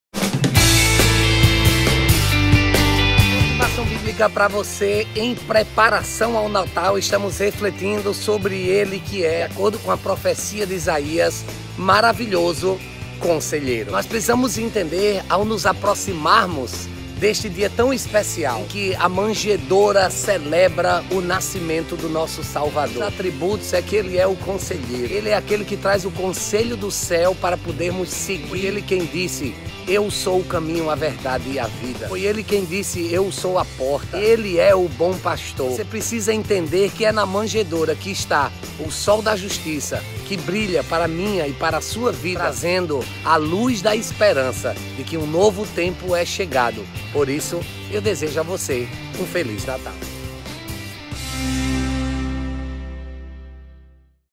Conselheiro
Se liga nesse vídeo com motivações bíblicas ministrado pelo Bispo Márcio Meira da Igreja Anglicana djp.anglicananobrasil.com e hoje pastoreia a anglicanacomunhao.com